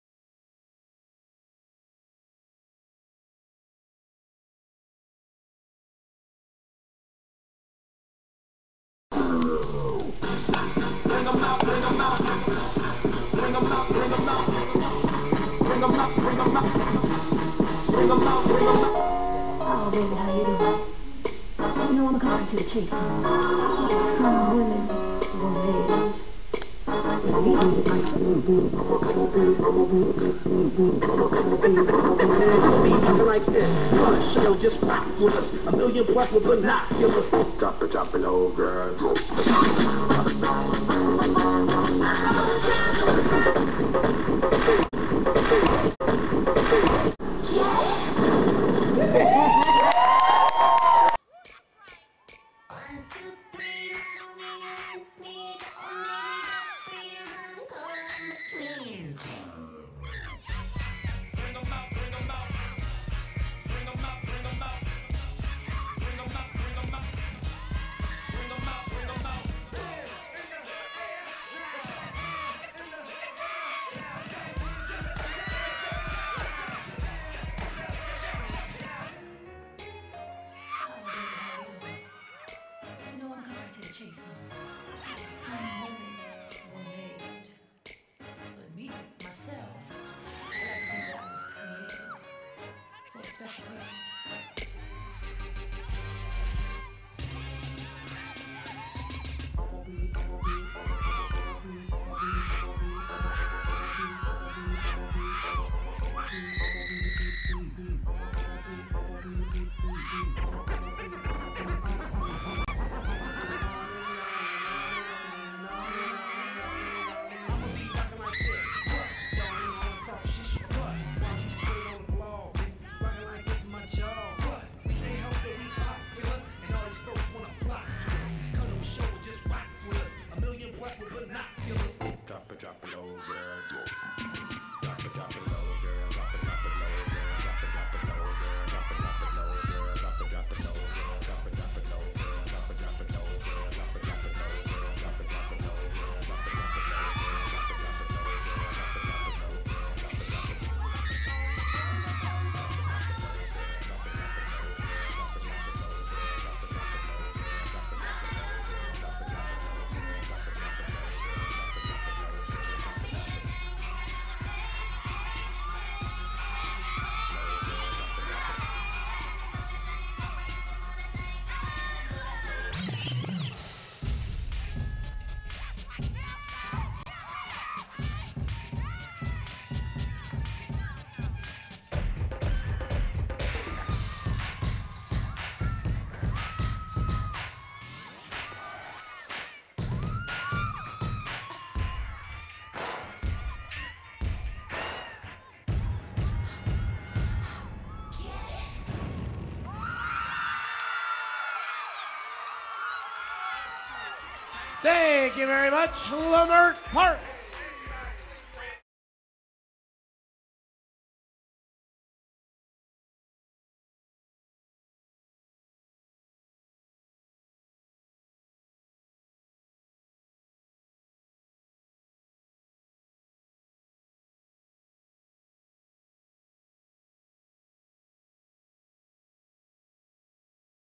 Leimert Park Chargers by Teangelo
This is a cheer squad I, Teangelo, choreographed into a first place win.